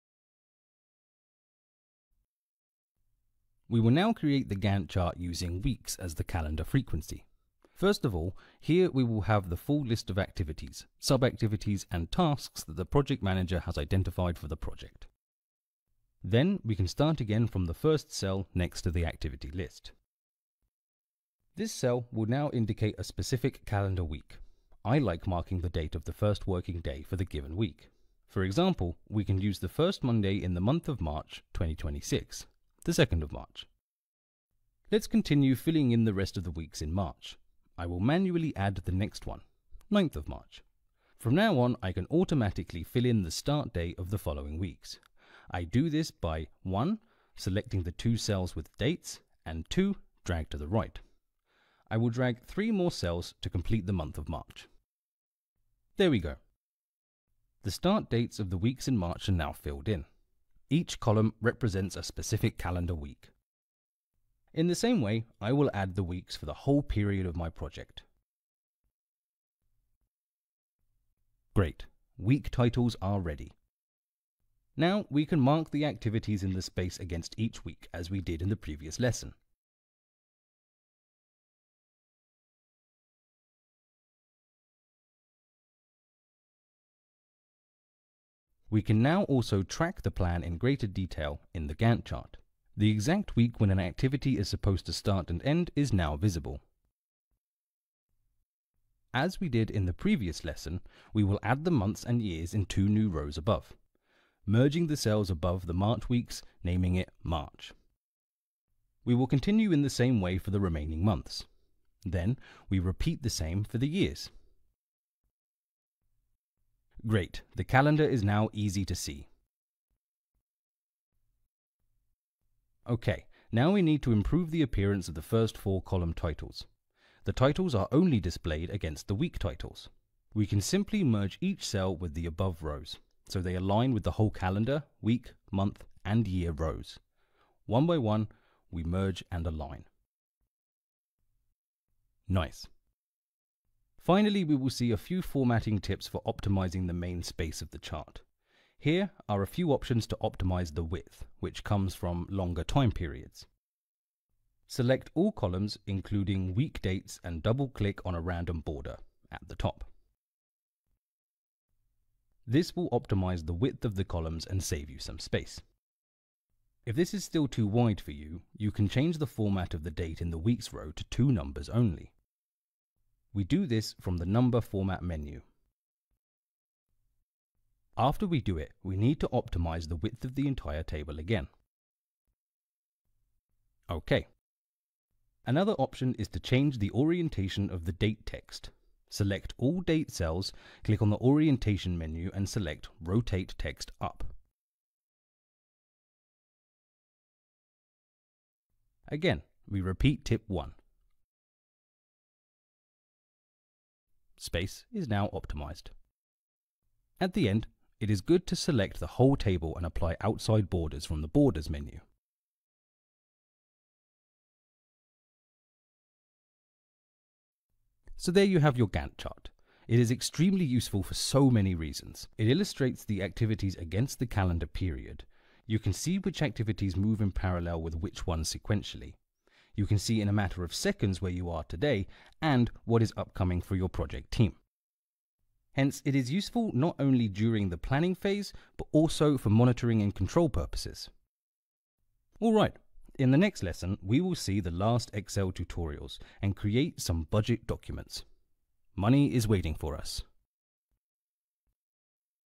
13. MS Excel for project management || Gantt Chart (weeks)
𝐌𝐒 𝐄𝐗𝐂𝐄𝐋 𝐅𝐎𝐑 𝐏𝐑𝐎𝐉𝐄𝐂𝐓 𝐌𝐀𝐍𝐀𝐆𝐄𝐌𝐄𝐍𝐓 𝐓𝐔𝐓𝐎𝐑𝐈𝐀𝐋

✅ This tutorial course will give you instructions how to apply MS EXCEL for Project Management work during all project phases and project lifecycle to manage the activities on your project with the most effective. The course will cover the content as follows:
✔ Tracking list
✔ Project management log
✔ Actions, Issues, Risk and Dependencies Log
✔ Project schedule, Gantt chart
✔ Budget form